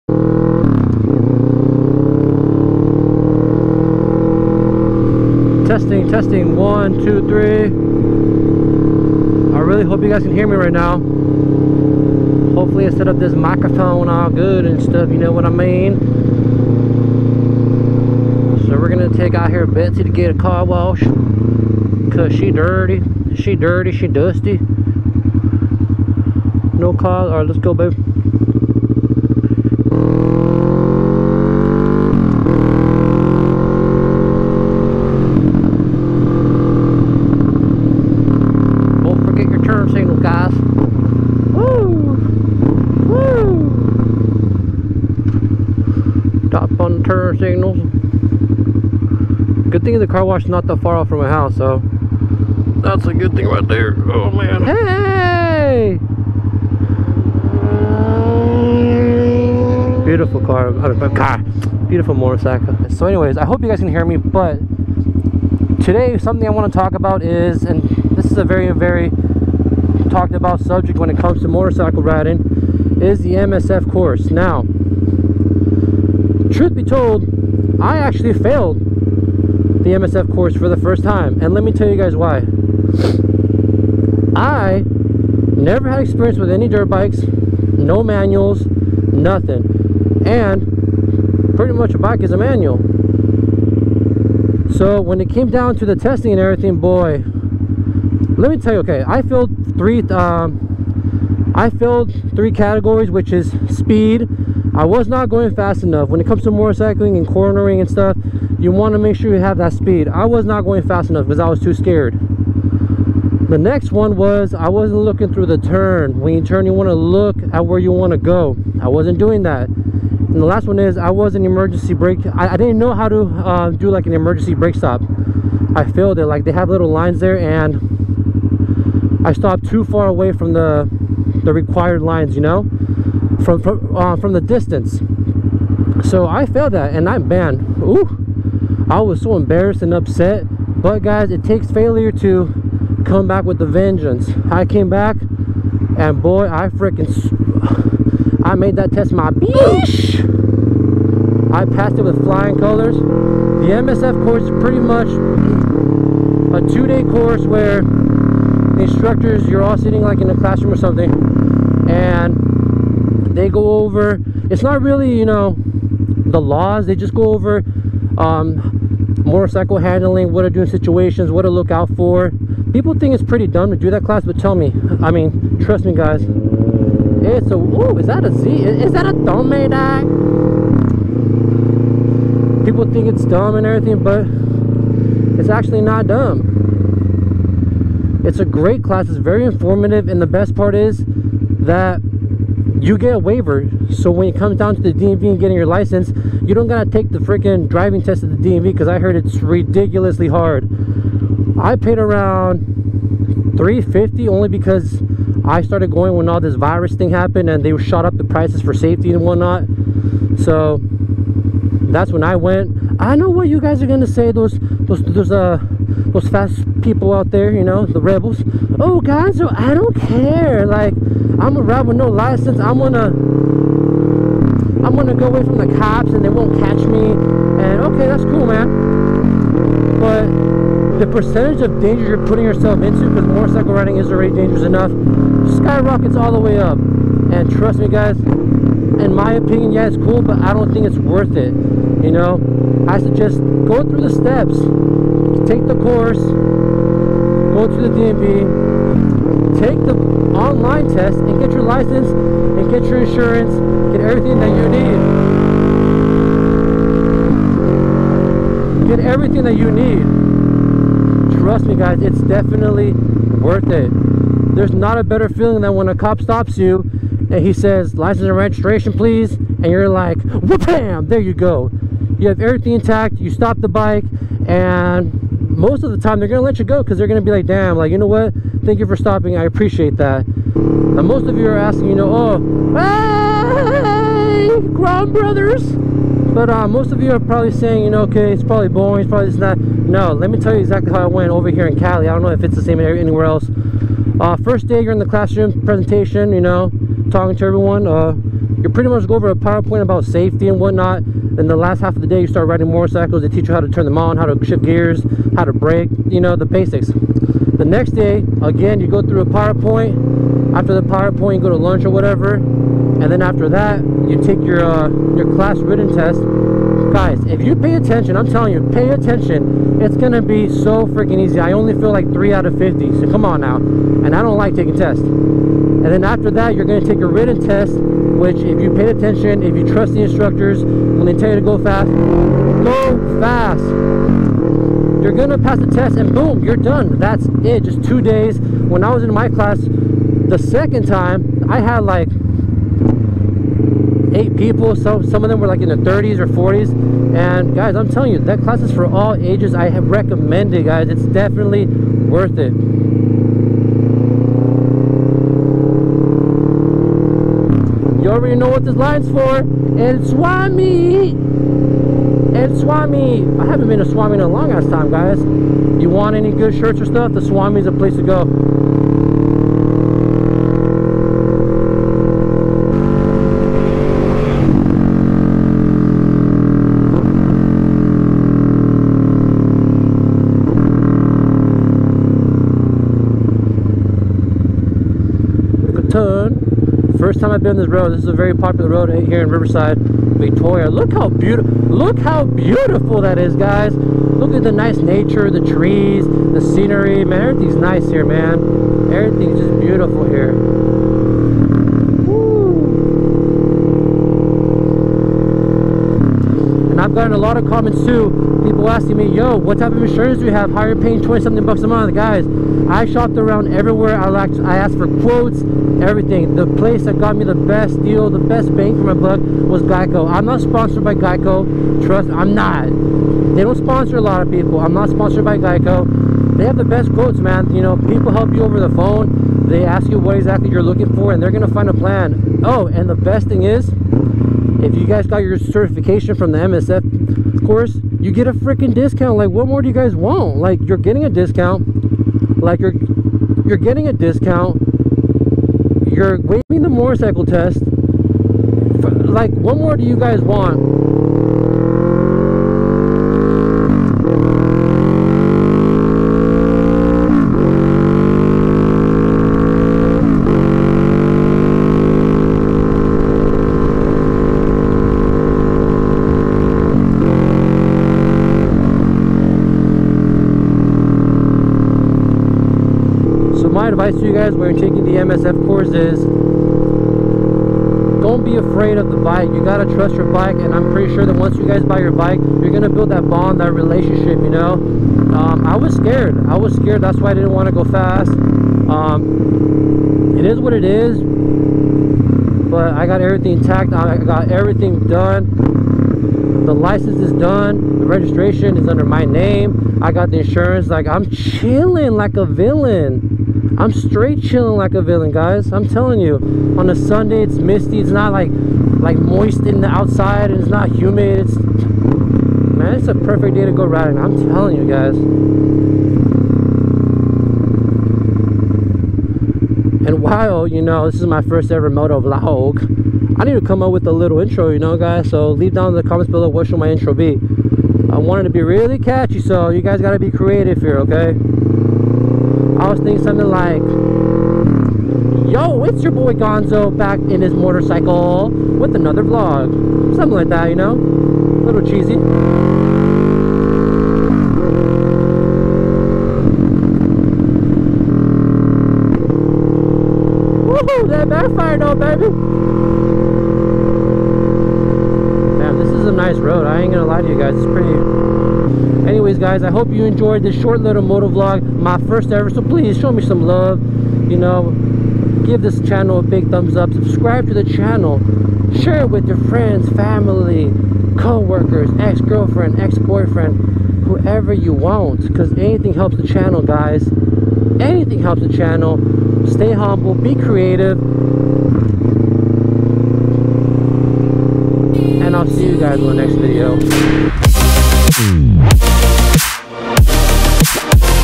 MOTOVLOG: I FAILED THE MSF COURSE!
Welcome guys to my first ever motovlog!
I hope you all enjoy it! Leave any questions you have below of what type of video I should make about motorcycles or any questions you have.